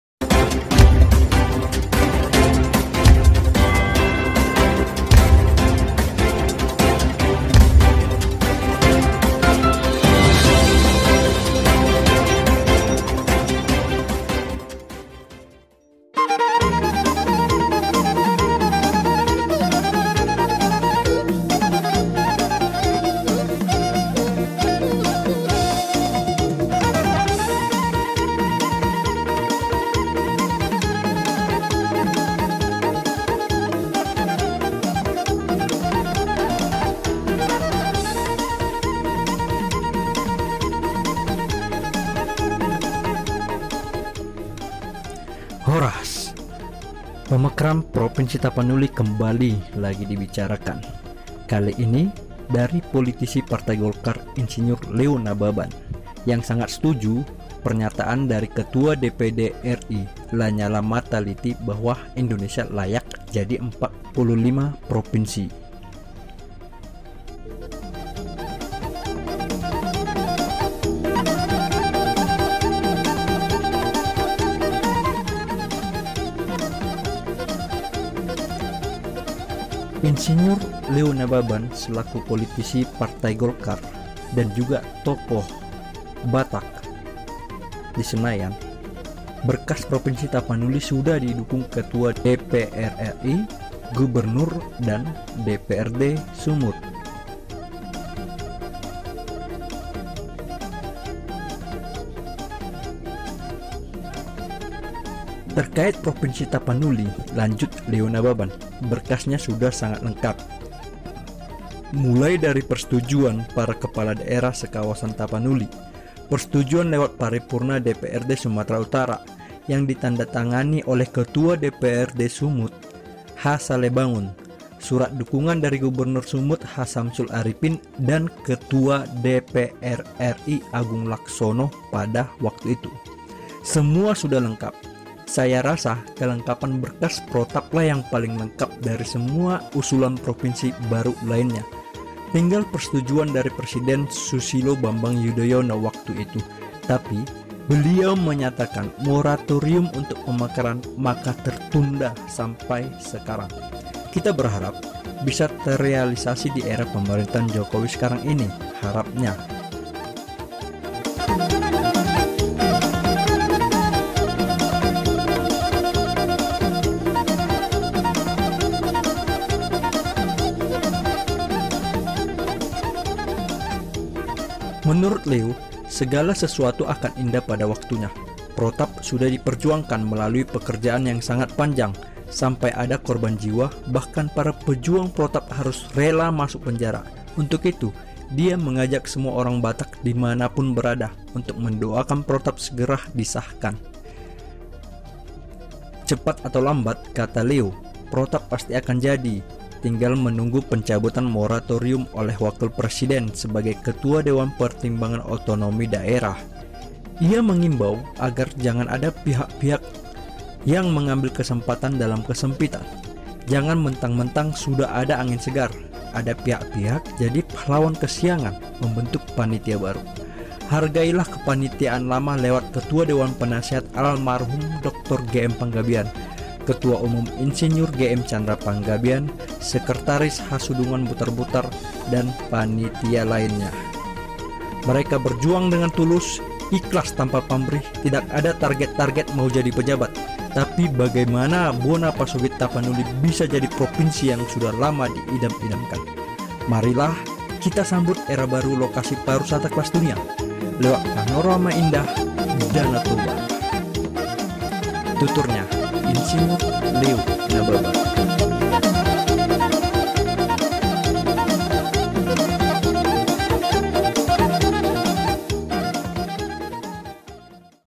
Provinsi Tapanuli || Ir.Leo Nababan - DPR RI, DPRDSU, GUBERNUR Sudah setuju
Berkas provinsi tapanuli paling lengkap diantara berkas pemekaran provinsi lainnya, Berkas provinsi tapanuli sudah didukung oleh DPR-RI, DPD-RI , DPRDSU , GUBERNUR juga Amanat Presiden  kata politisi golkar dan mantan staf ahli DPR-RI Agung Laksono

Dia menghimbau agar tidak ada mencari kesempatan atau membentuk panitia-panitia baru karena berkas protap sudah lengkap dan tinggal disetujui oleh pemerintah dan moratorium dicabut provinsi Tapanuli terbentuk.

Kreativ commons

Mohon bantuan doanya agar perjuangan masyarakat Batak yang selama ini juga dinanti-nantikan dapat terwujud dalam waktu dekat.